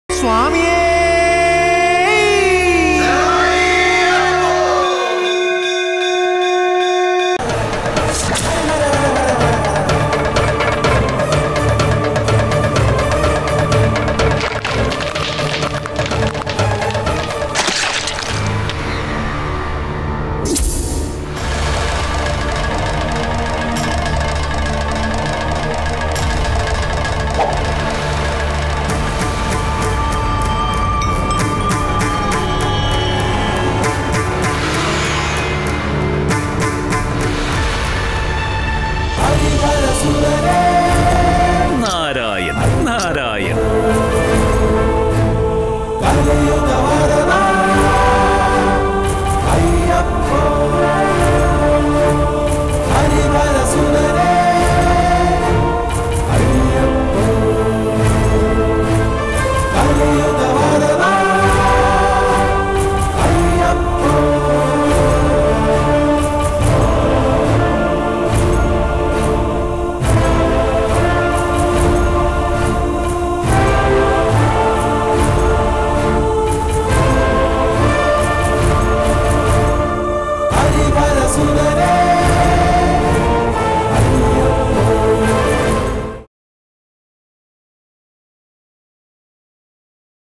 തീപാറുന്ന യുദ്ധത്തിനൊടുവിൽ മഹിഷിയെ നിഗ്രഹിച്ച് മണികണ്ഠൻ!
തീപാറുന്ന യുദ്ധത്തിനൊടുവിൽ മഹിഷിയെ നിഗ്രഹിച്ച് മണികണ്ഠൻ! അനുഗ്രഹവർഷം ചൊരിഞ്ഞ് ത്രിമൂർത്തികളും ദേവലോകവും..